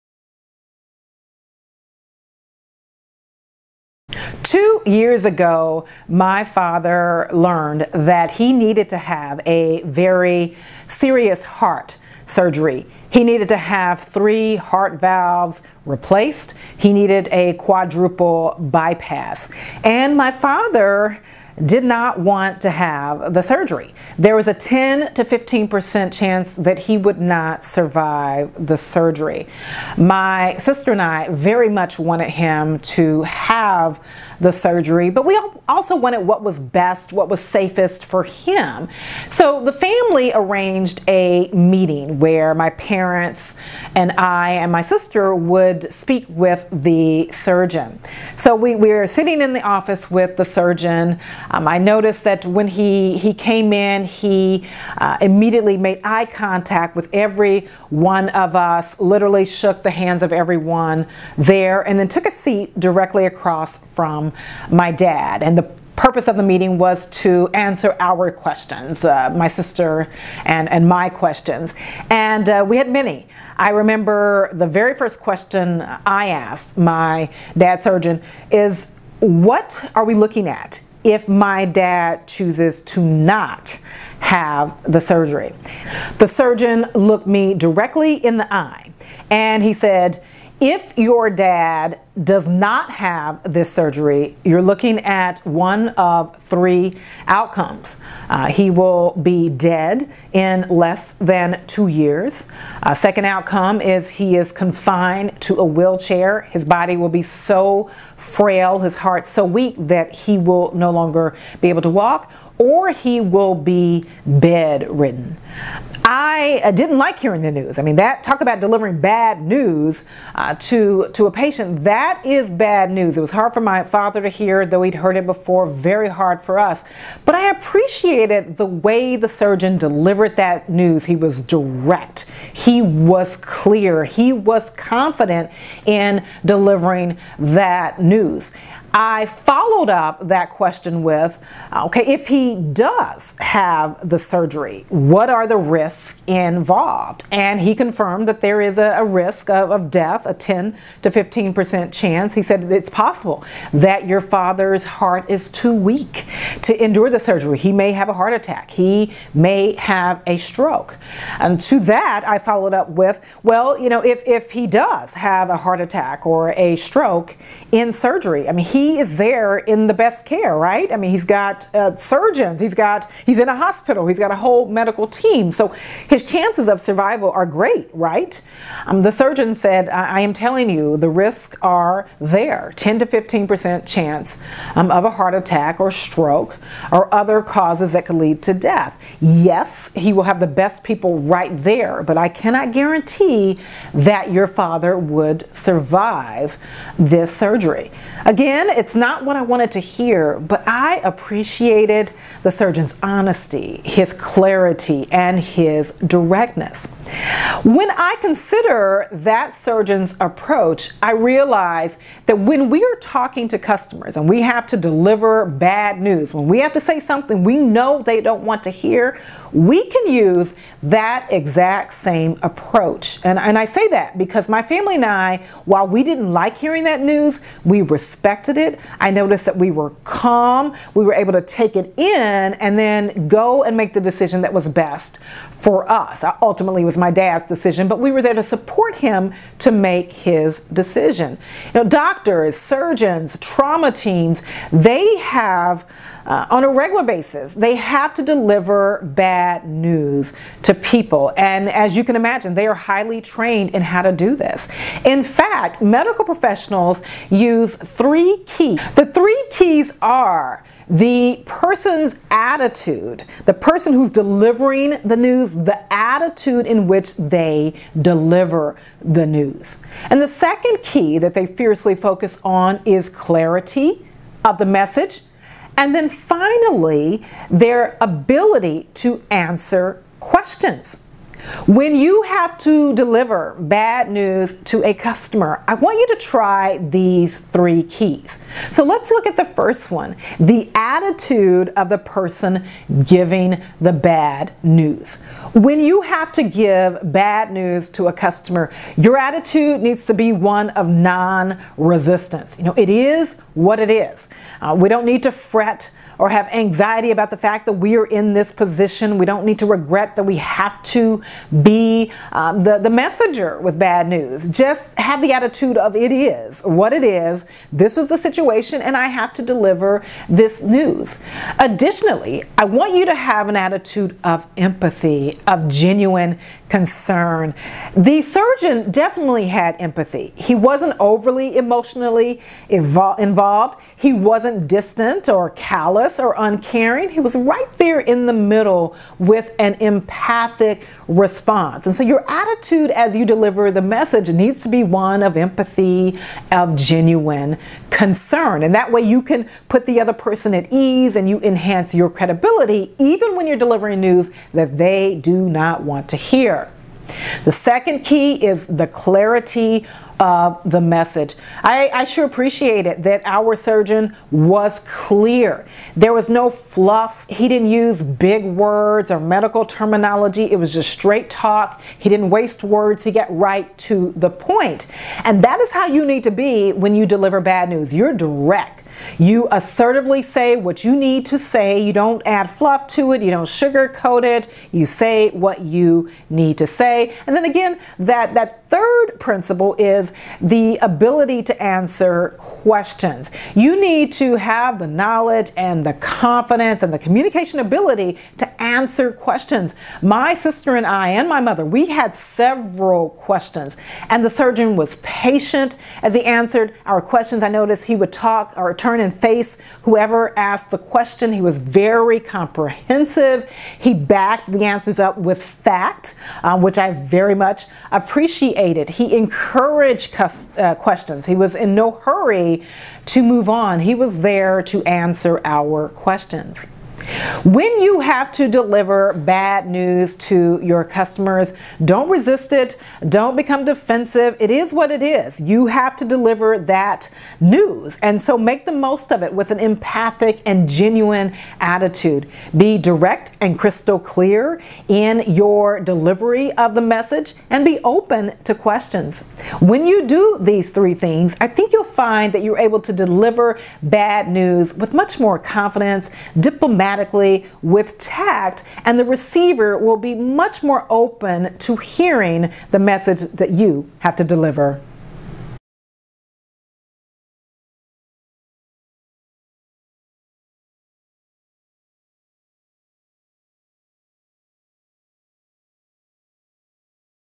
How to Deliver Bad News to a Customer
This video is about how to deliver bad news to customers and presents 3 keys that doctors use to deliver bad news to patients and their families.